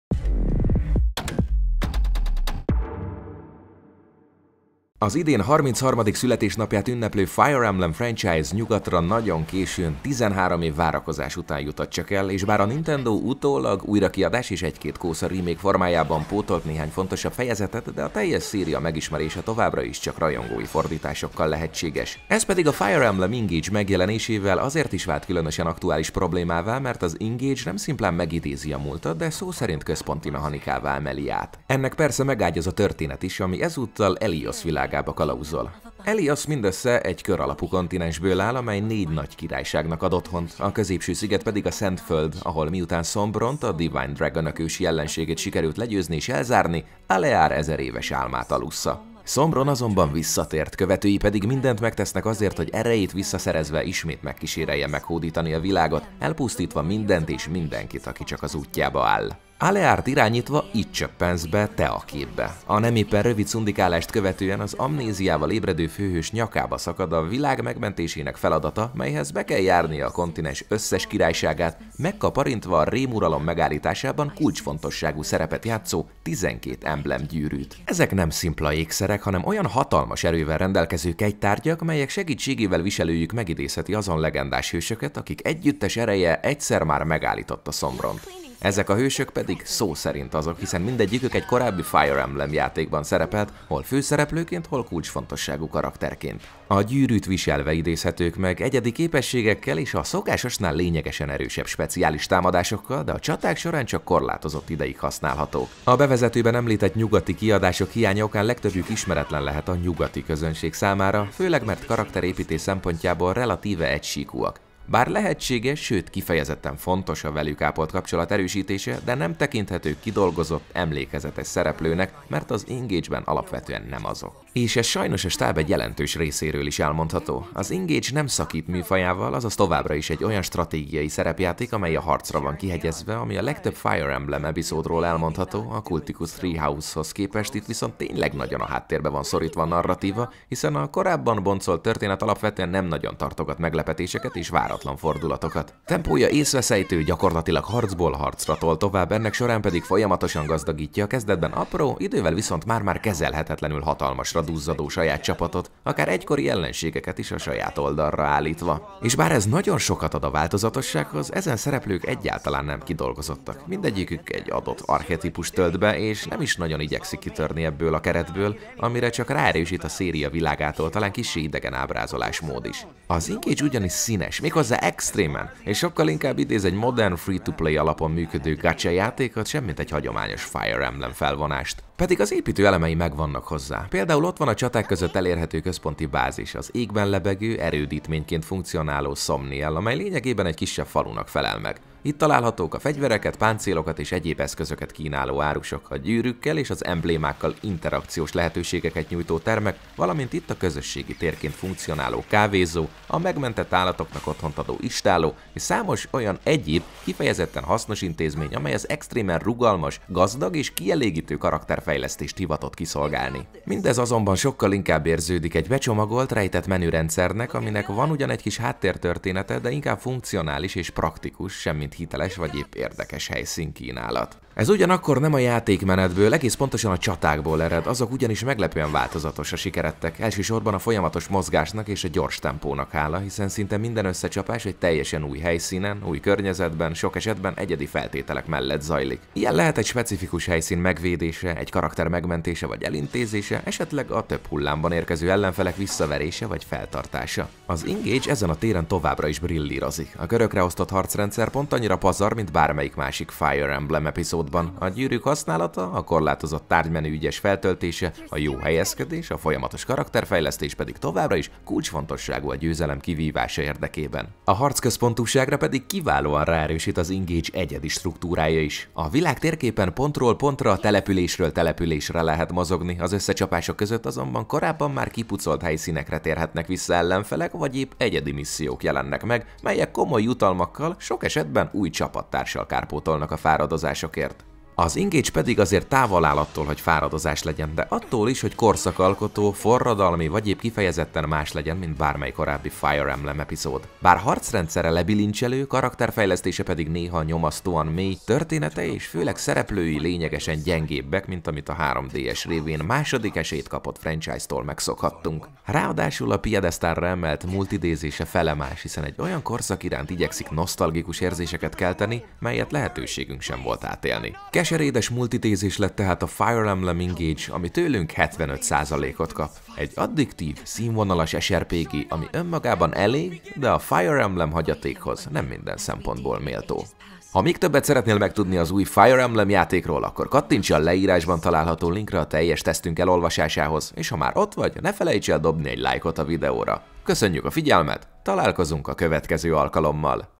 A gyűrűk ura ► Fire Emblem Engage - Videoteszt
Szöveg: wilson
Narráció: TZeus
Vágó: Pizza

A múltat és a jelent igyekszik fuzionálni a legújabb Fire Emblem-felvonás, amely harminc év tapasztalatait és mérföldköveit önti a már jól ismert keretbe.